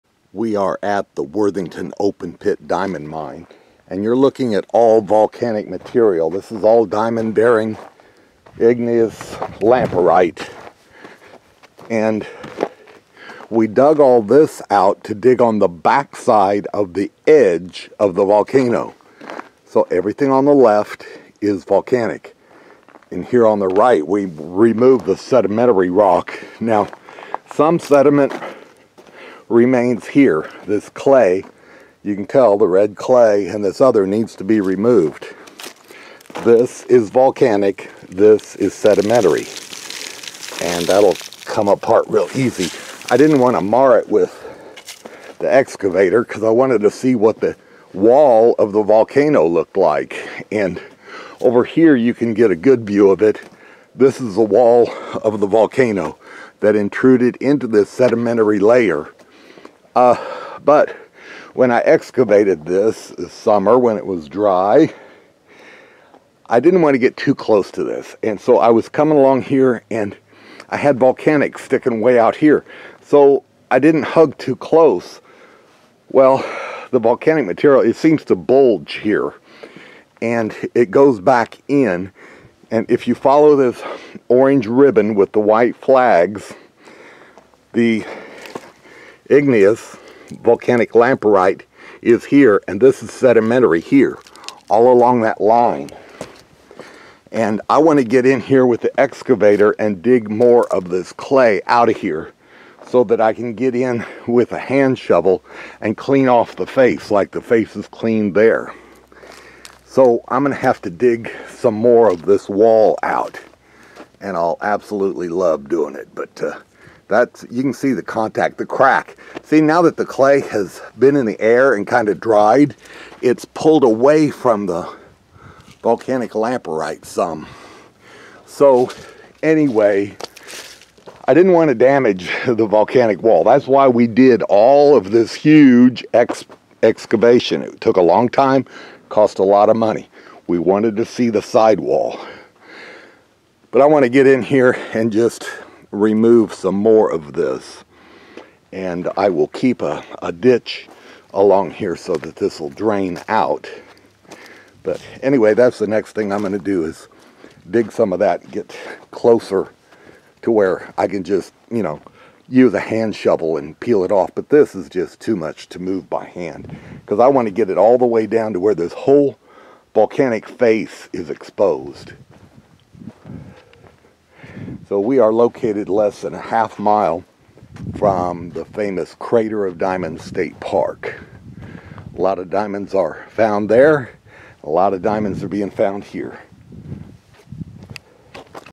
We Are Examining The Edge (The Side Wall) Of The Diamond-Bearing Volcanic Intrusion At Our Mine
Some of the volcanic side wall has been exposed for us to study. Now, we have a little more that needs to be carefully removed so we don't scar the igneous, diamond-bearing vent material.